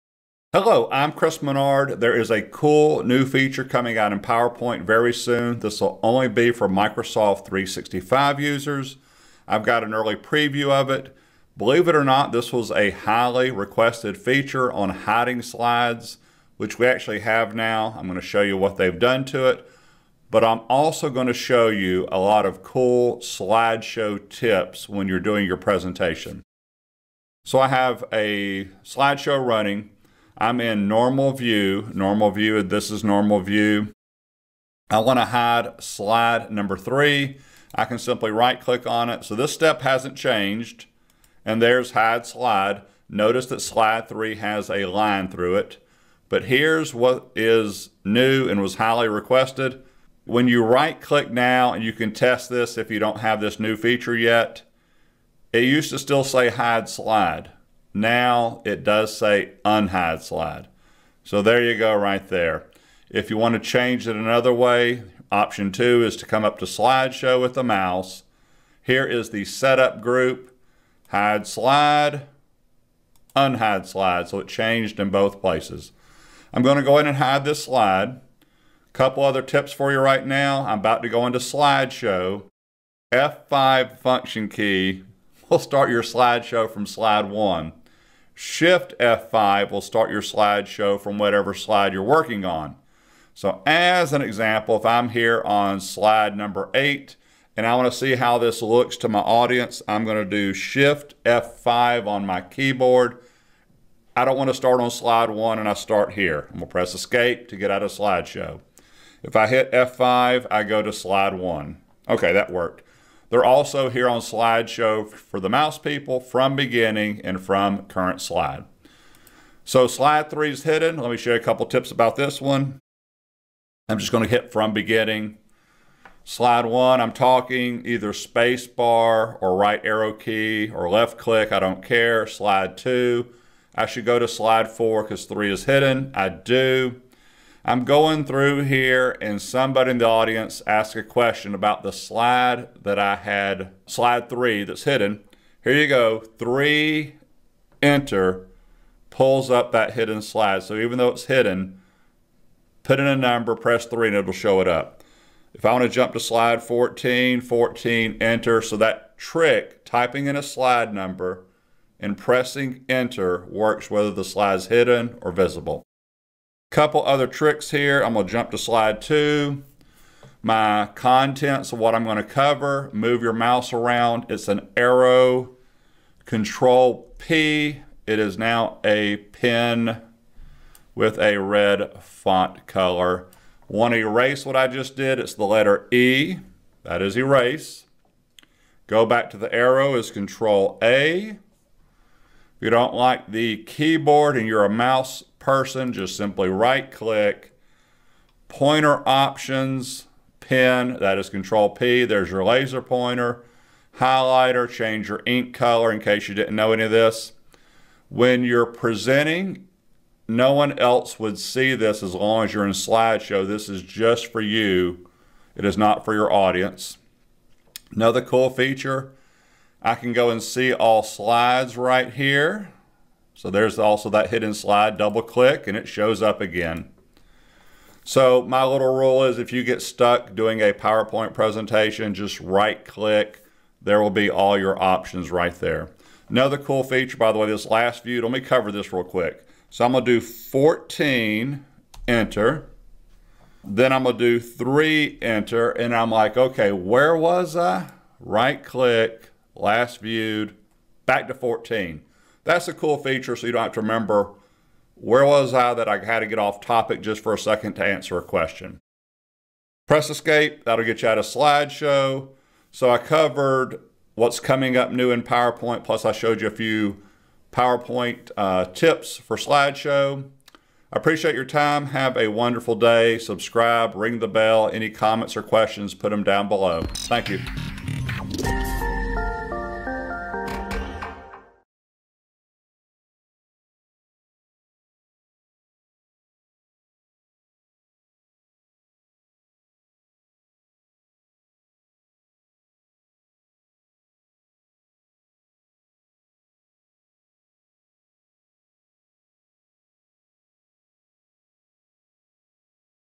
PowerPoint Hide & Unhide slides now have different commands and Slideshow Tips
In PowerPoint, it was unclear how to unhide a slide. Previously, when you wanted to hide a slide, you right-clicked the slide and selected Hide. But to unhide the slide, the command still showed Hide instead of Unhide. Now, the command for unhiding is correctly labeled Unhide Slide.

Chapters / Bookmarks
0:00 Intro
0:25 Hide slide
0:50 Unhide Slide
1:22 Keyboard shortcuts for Slide Show
3:58 Last viewed

Keyboard shortcuts
F5 to run the slideshow on slide 1
Shift + F5 Run the slideshow from the current slide
CTRL + P Pen
CTRL + A Arrow
Type slide number and press Enter to go to a slide
ESC to get out of Slide Show

Hide / Unhide is also located on the Slide Show Tab in the Set Up group.

-- EQUIPMENT USED
○ My camera